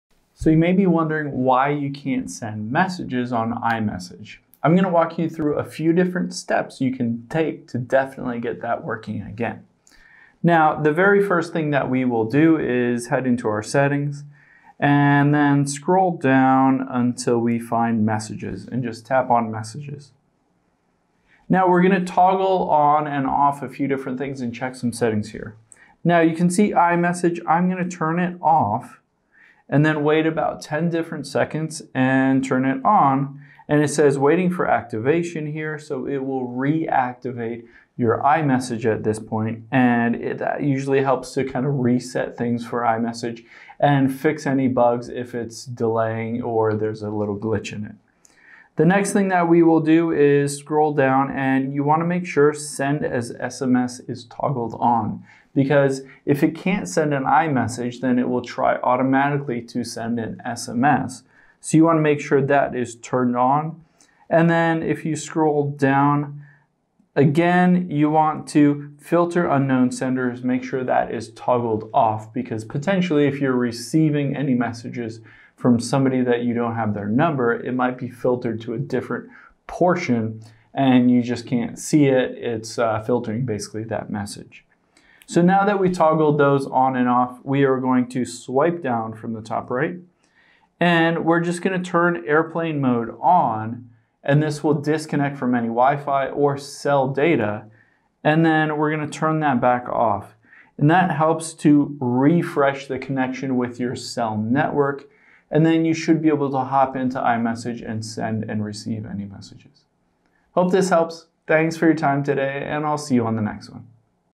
Why Can't I Send Messages On iMessage?
Let's go over why you can't send any text messages to your friends on iMessage and how to fix it.